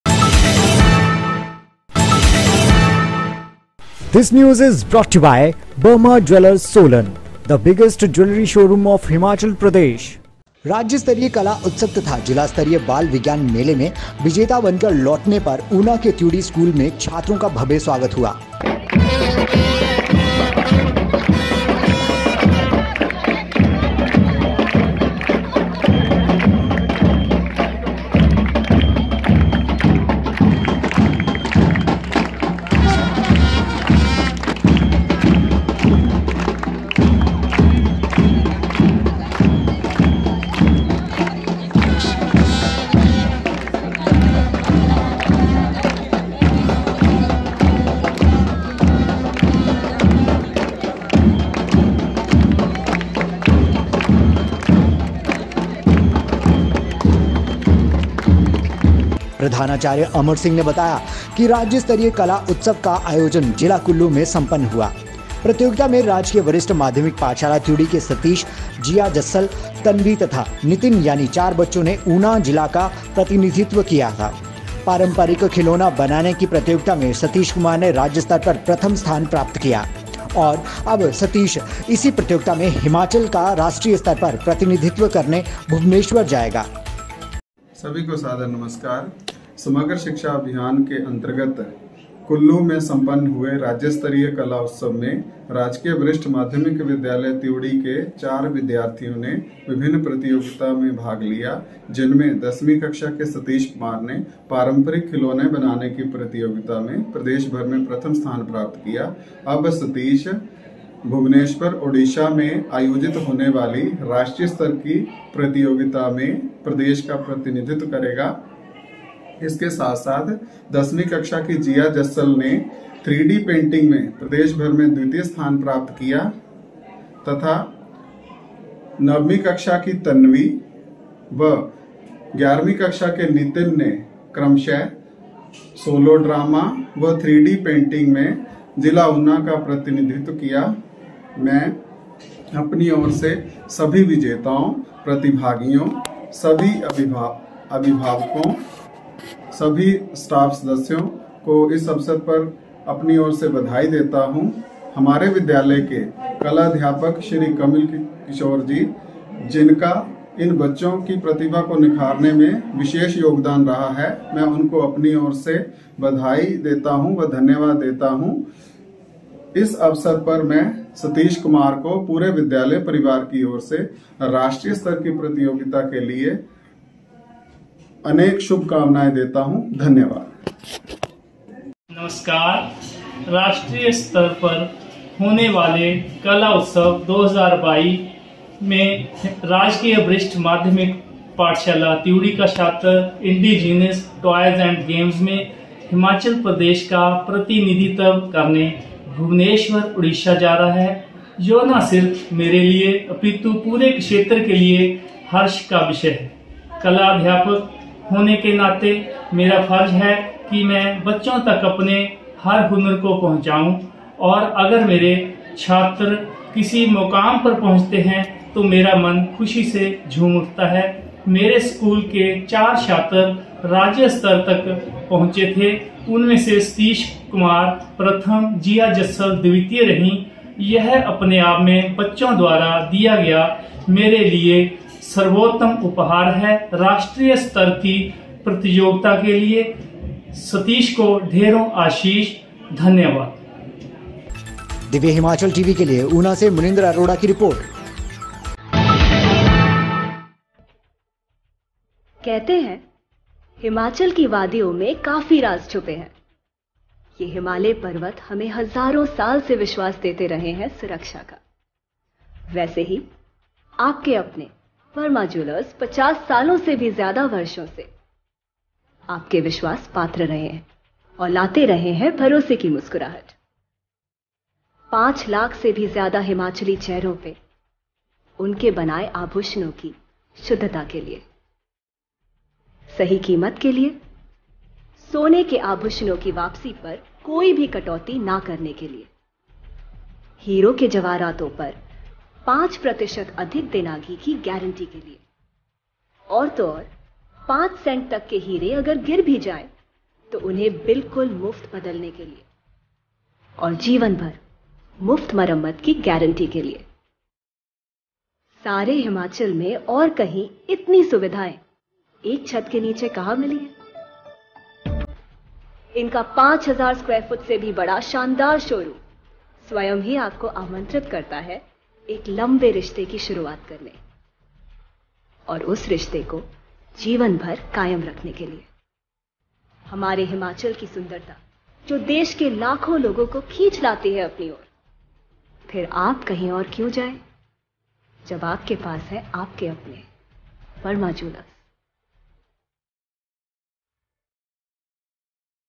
त्यूड़ी स्कूल का सतीश करेगा राष्ट्रीय स्तर पर कला उत्सव में हिमाचल प्रदेश का प्रतिनिधित्व
HimachalNews: त्यूड़ी स्कूल का सतीश करेगा राष्ट्रीय स्तर पर कला उत्सव में हिमाचल प्रदेश का प्रतिनिधित्व
divyahimachaltv
dhtv,tvnews
local news
divyahimachalgoodmorning
khabre ftafat
divyahimachalgoodafternoon